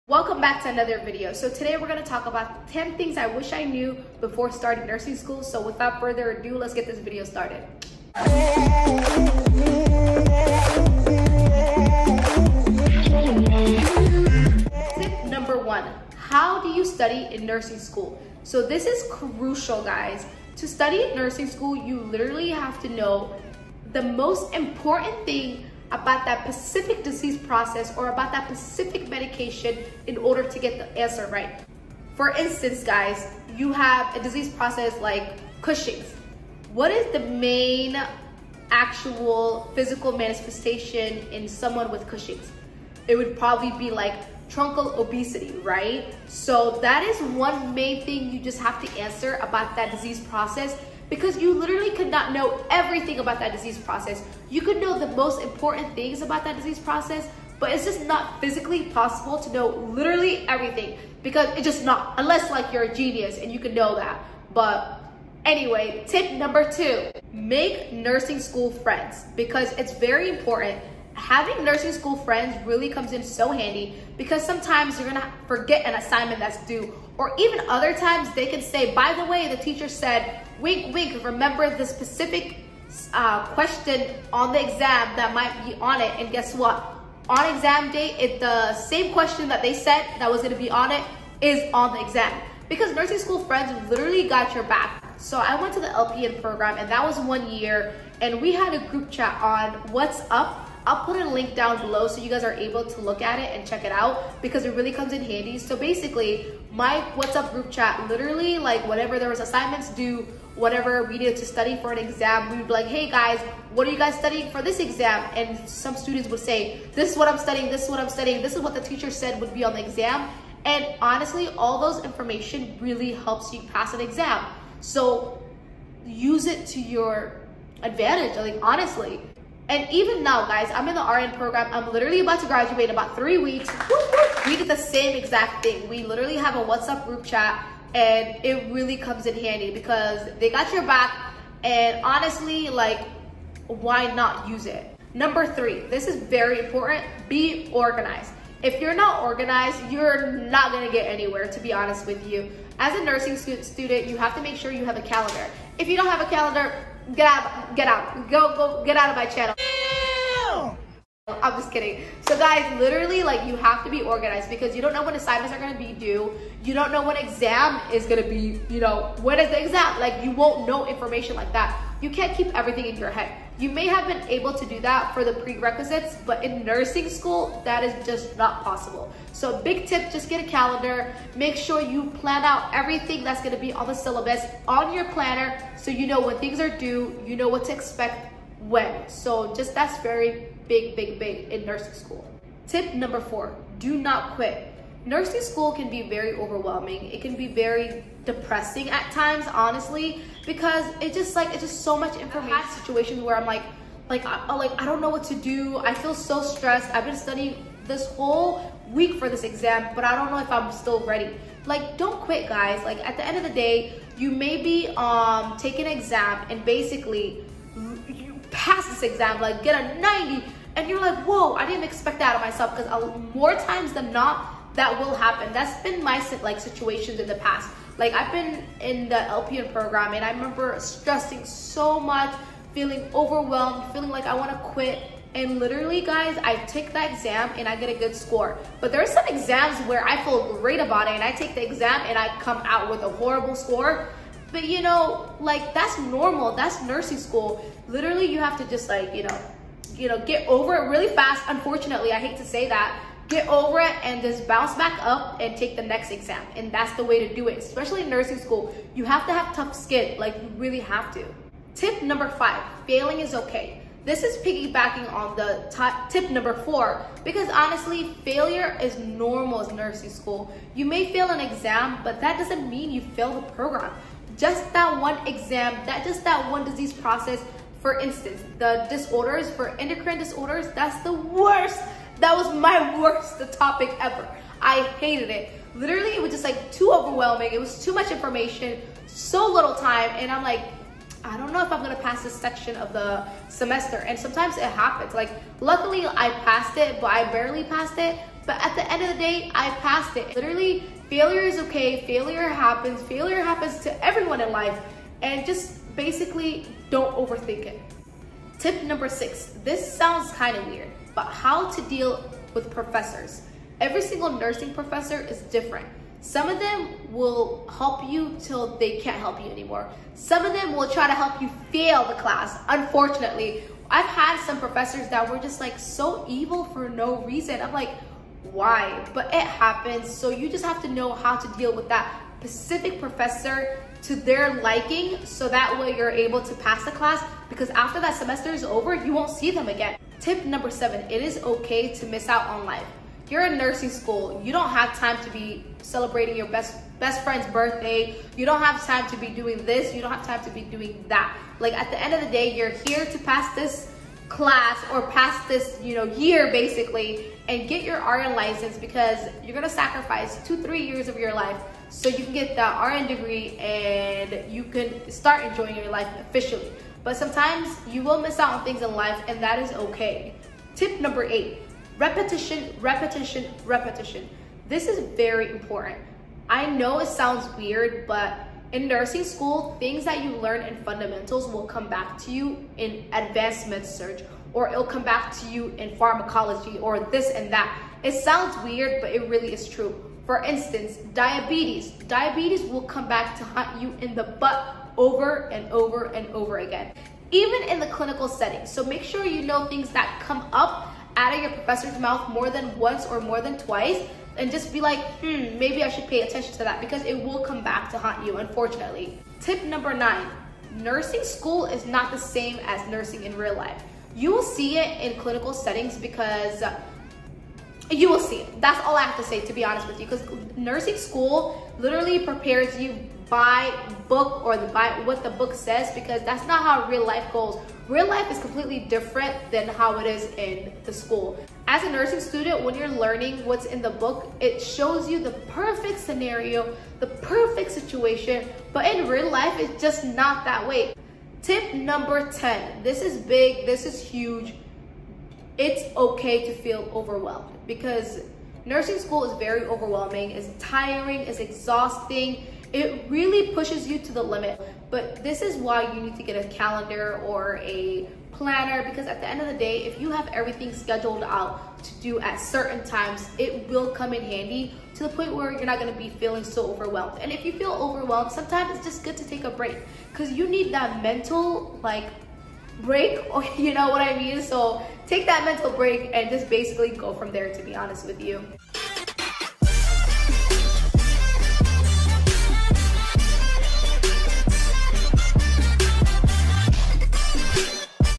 10 THINGS I WISH I KNEW BEFORE STARTING NURSING SCHOOL
Hello lovely people!  Today we will be talking about 10 things I wish I knew before starting nursing school with tips and tricks.